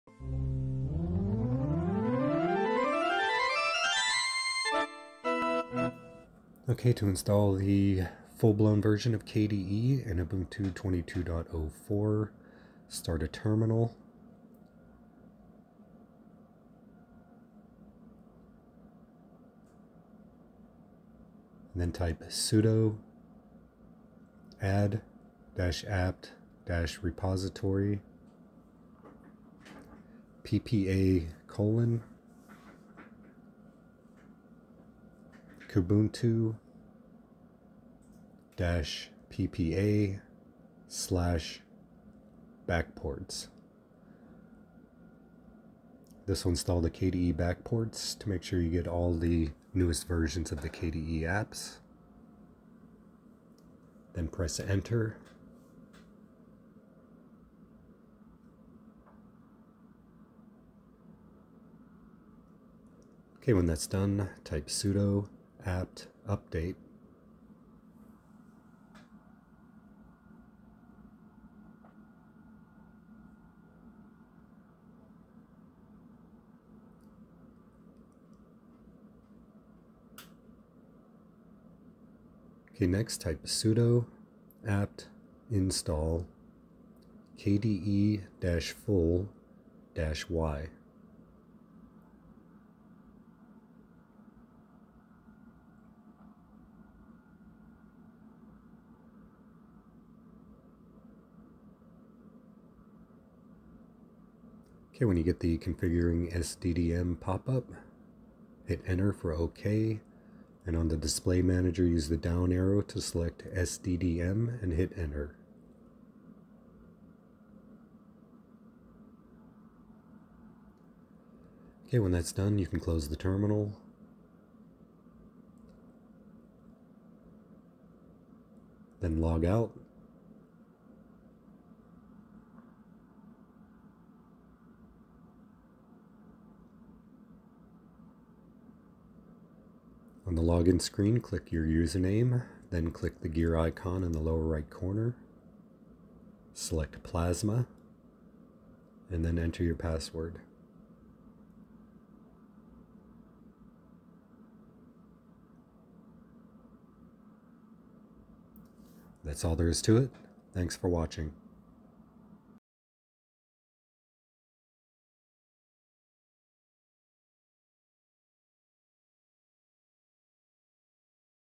Install KDE Plasma Desktop In Ubuntu 22.04 & 23.04
How to install KDE Plasma Desktop In Ubuntu 22.04 & 23.04.